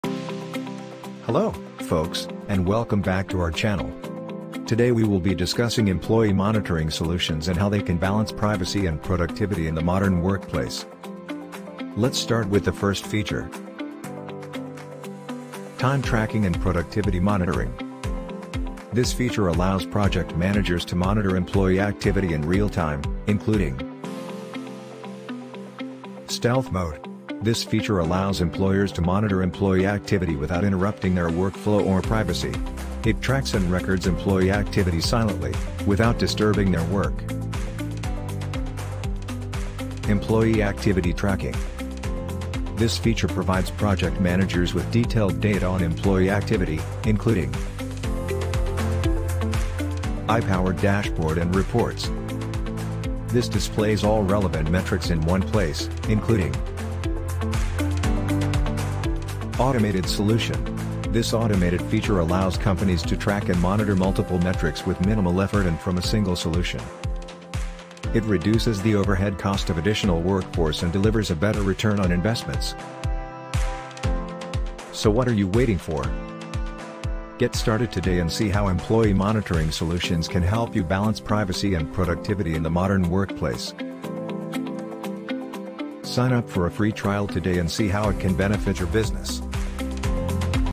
Employee Monitoring Solutions: Balancing Privacy and Productivity in the Modern Workplace
In today's digital age, employee monitoring has become a hot topic for businesses of all sizes.

While monitoring software can help employers improve productivity and security, it also raises concerns about employee privacy.

This video explores the delicate balance between privacy and productivity in the modern workplace and the role of employee monitoring solutions in maintaining this balance with its different features
1) Time Tracking
2) Productivity Monitoring.
3) Stealth Mode
4) Employee Activity Tracking
5) Ai-Powered Dashboard And Reports
6) Automated Solution

Watch the complete to know how employee monitoring software can help you to balance privacy & productivity in the modern workplace.